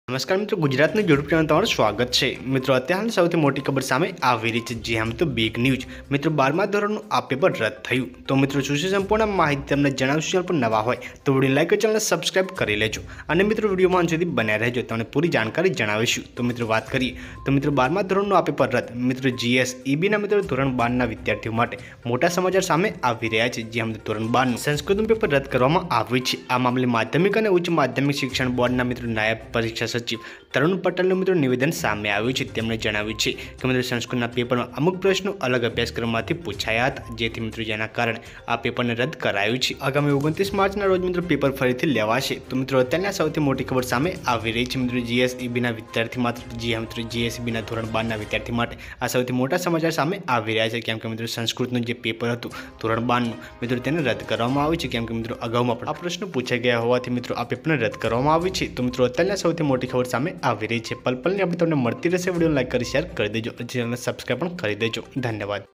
28 માર્ચ 2023 આજની મોટી ખબર | રાજ્યનાં મુખ્ય સમાચાર | 28 March 2023 today Taza samachar paper rad
25 માર્ચ 2023 આજની મોટી ખબર | રાજ્યનાં મુખ્ય સમાચાર | 25 March 2023 today Taza samacharપરંતુ અમે તેમના ષડયંત્રને ક્યારેય સફળ થવા દઈશું નહીં. કેજરીવાલ લોકોના અધિકાર માટે ખડકની જેમ ઉભા જોવા મળશે.LG સાહેબ, બાદમાં મહેરબાની કરીને એવું ના બોલો કે મર્યાદા તોડાઈ રહી છે.પછી કહેવાય છે કે કેજરીવાલ બહુ લડે છે. દિલ્હીની મફત વીજળી રોકવા માટે ષડયંત્ર રચવામાં આવી રહ્યું છે,દિલ્હીમાં મુખ્યમંત્રી અરવિંદ કેજરીવાલ અને LG વચ્ચે ફરી વિવાદ થયો છે. કેજરીવાલે ટ્વીટ કર્યું કે,